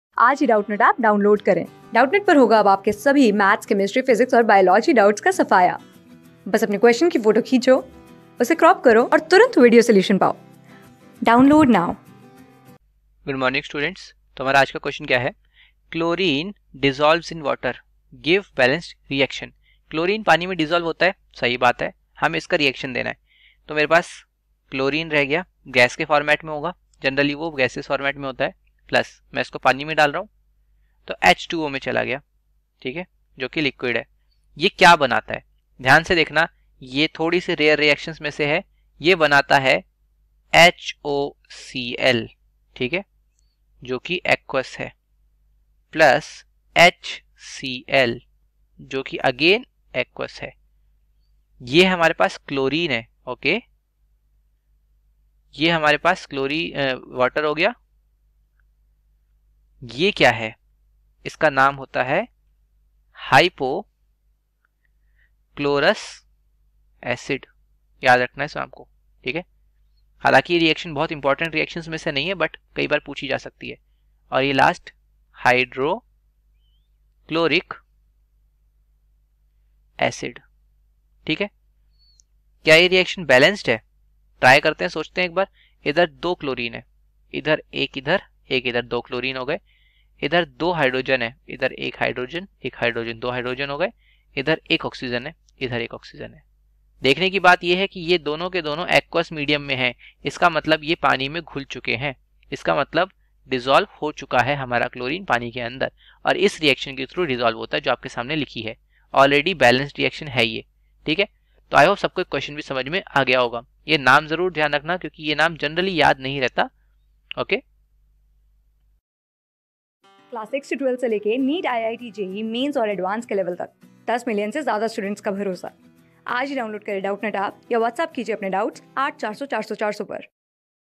Chlorine dissolves in water. Give balanced reaction. | CLASS 10 | BALANCED CHEMICAL EQUATIONS
Chlorine dissolves in water. Give balanced reaction.
Class: 10
Subject: CHEMISTRY
Chapter: BALANCED CHEMICAL EQUATIONS
Board:Maharashtra Board

👉 Have Any Query? Ask Us.